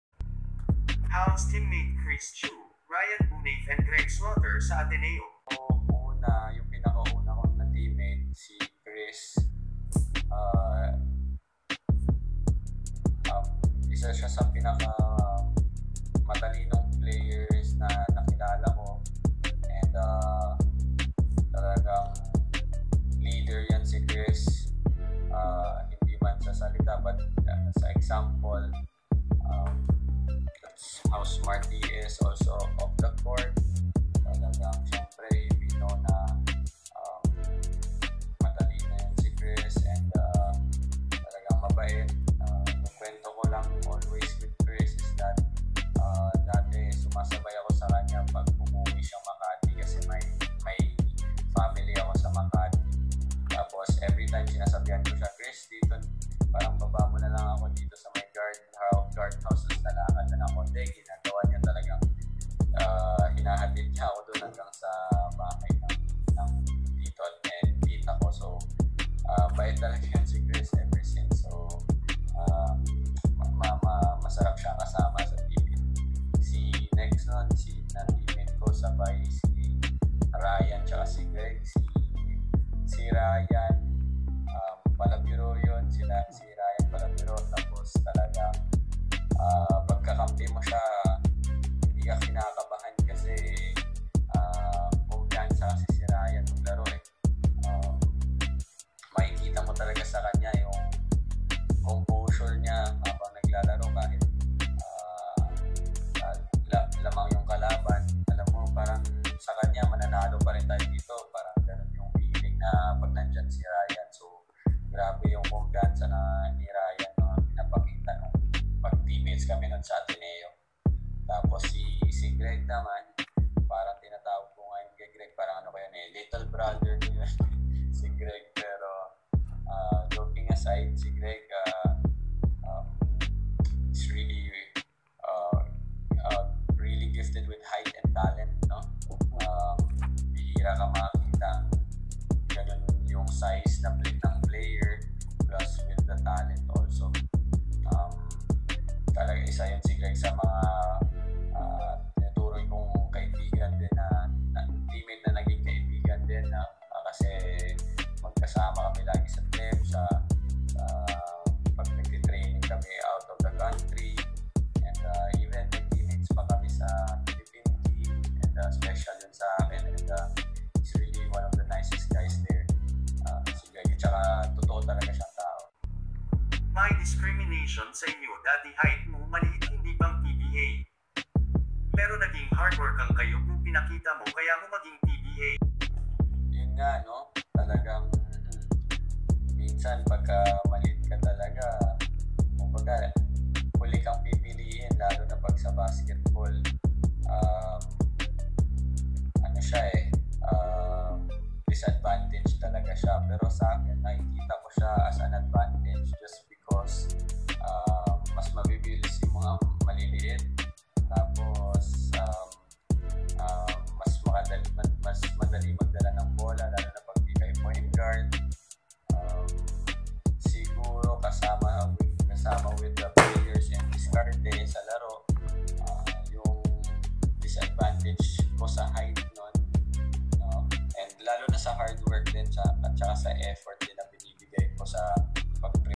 Emman Monfort shared experience playing ateneo teammates Tiu,Slaughter and Ryan| Ep4 Emman Monfort
Emman Monfort shared experience ateneo teammates Chris Tiu,Slaughter and Buenafe| Ep4 Emman Monfort